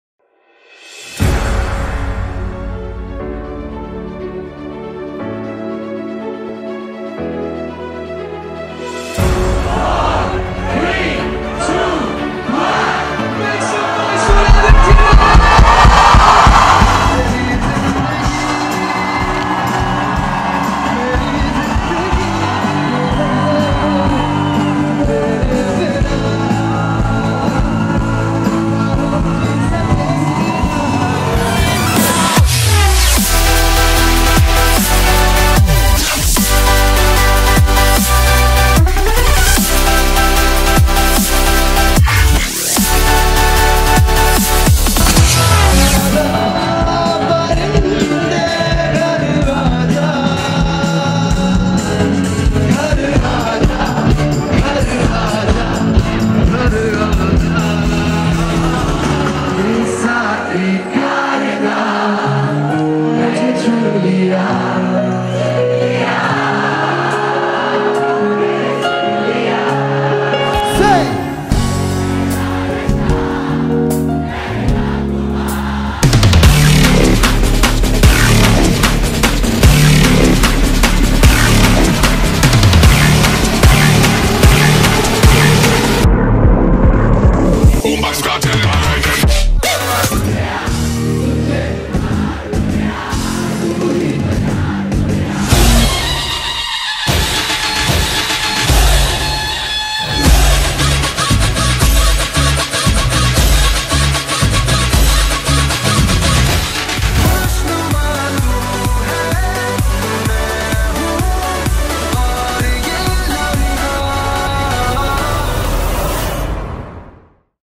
Aditya Narayan | Live in Concert | Mood Indigo - IIT Bombay | STARtist Management
Experience STARtistexclusive ADITYA NARAYAN’s rocking and energetic live performance !

Singer-STARtistexclusive Aditya Narayan